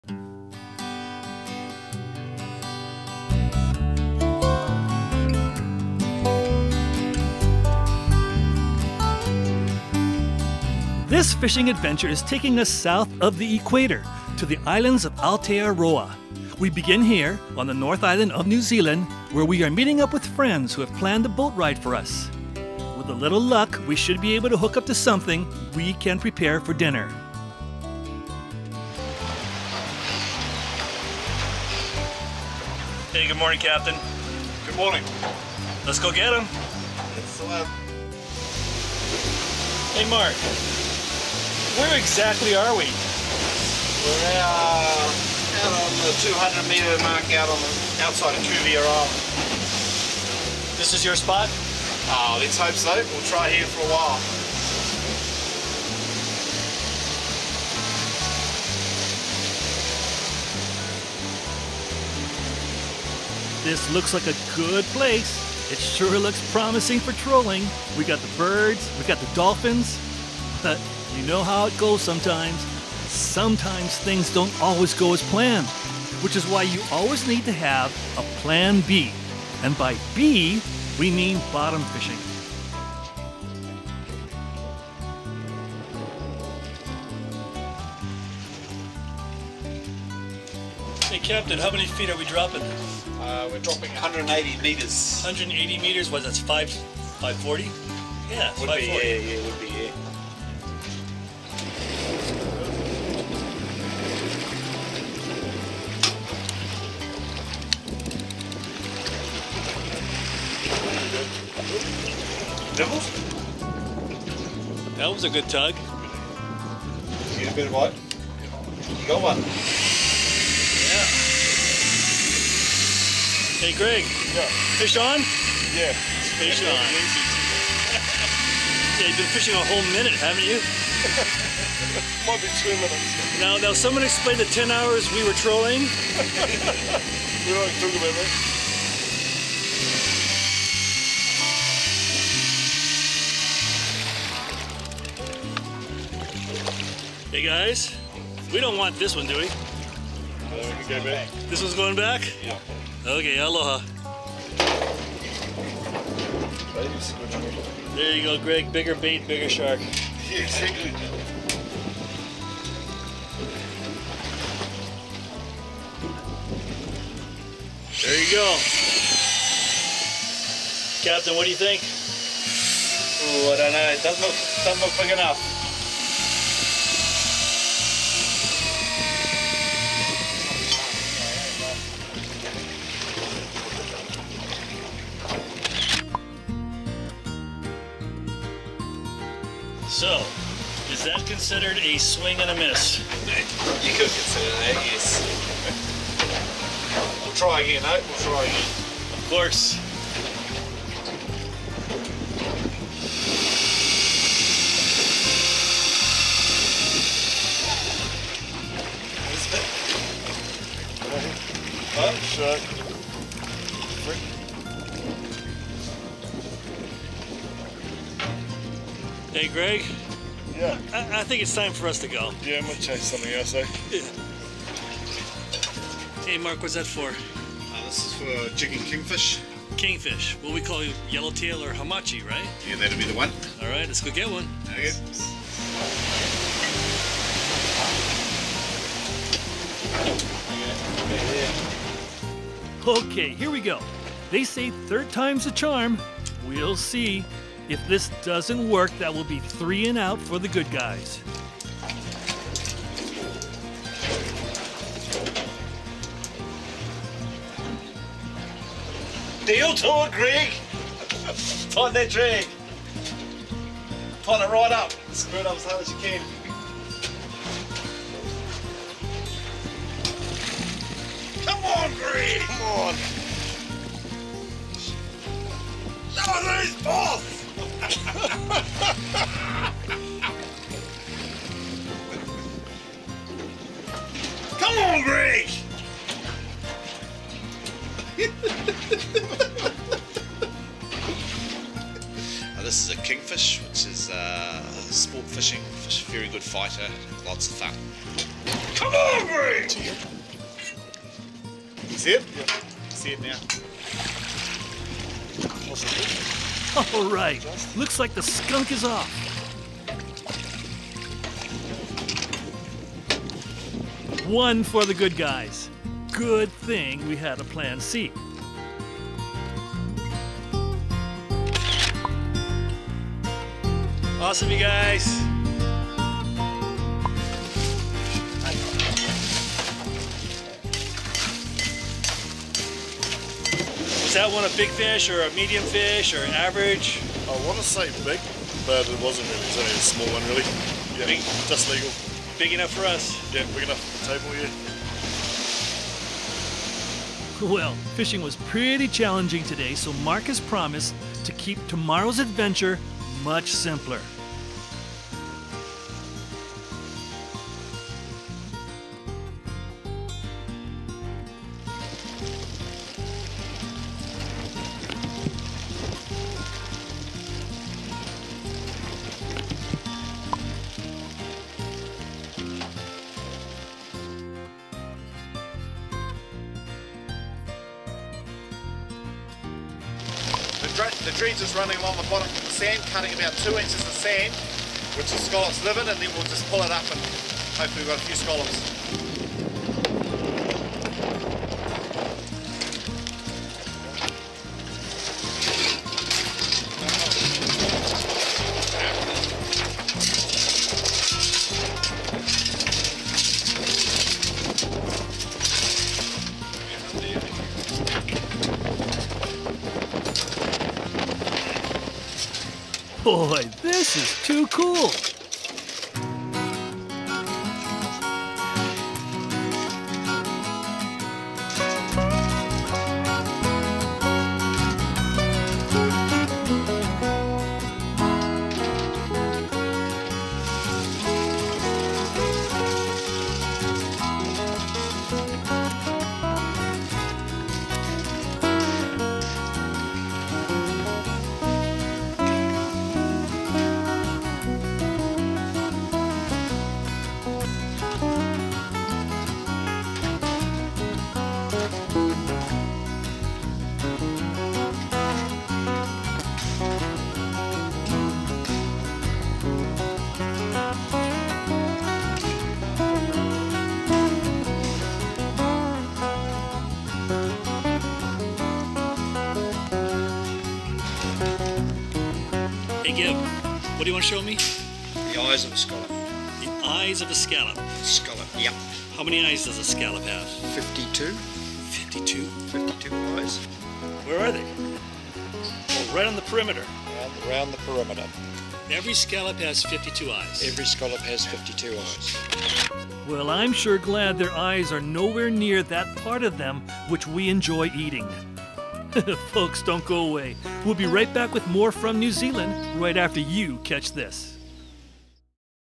LGF 175 Pt. 1: New Zealand Kingfish And Scallops Safari!
FEATURED LOCATION:  Coromandel, North Island.
FEATURED SPECIES:  Shark, Scorpionfish, Yellowtail, Scallops.

Few destinations are as enjoyable as New Zealand to visit!  Both North Island and South Island here offer an amazing variety of adventures!

On this trip we spend a couple of days fishing with Mark Bull and Greg Somerville.  The first day, after a few false starts with sharks and scorpionfish, we manage to jig up a "keeper" Kingfish (Yellowtail/Hamachi).

The next day we enjoyed a much simpler activity.  Mark took us out to harvest scallops!  Then we get schooled on how to clean and prepare these delicious shellfish for dinner!

INVITE US!